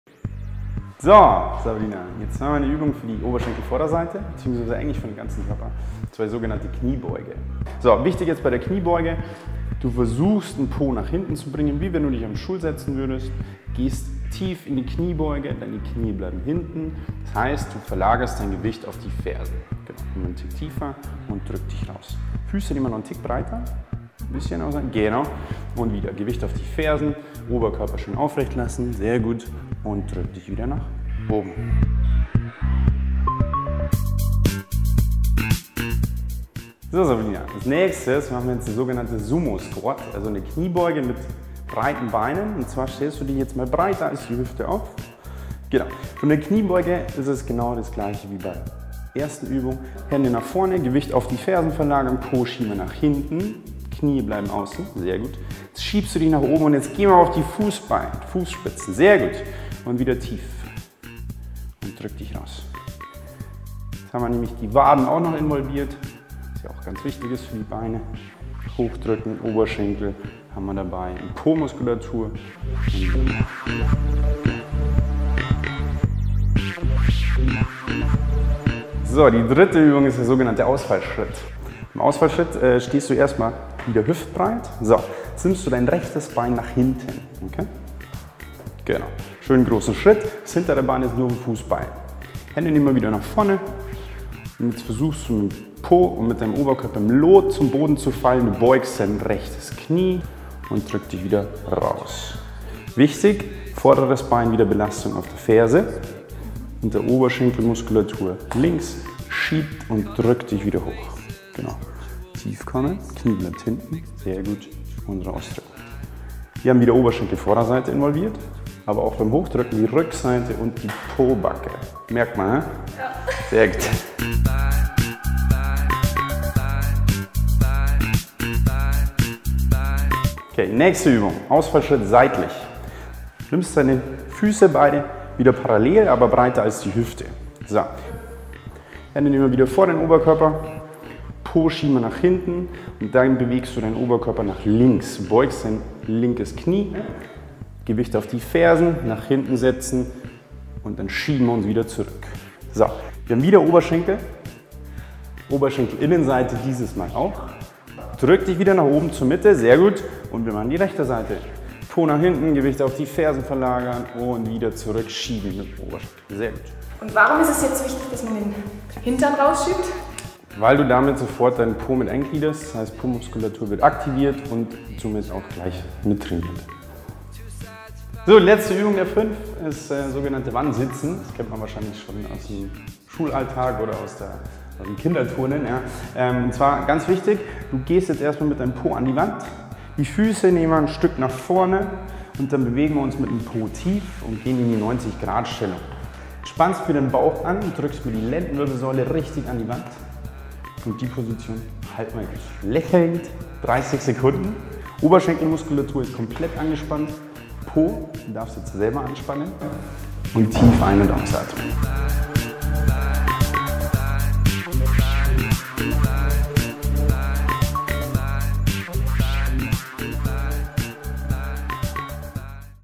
Mit diesen Übungen passt die Lieblingsjeans endlich perfekt
- Traumhafte Beine sind mit diesen fünf einfachen Übungen kein Luxus mehr. FOCUS Online Reporterin Sabrina Nickel zeigt ein simples Workout, nach dem Eure Lieblingsjeans endlich perfekt sitzt.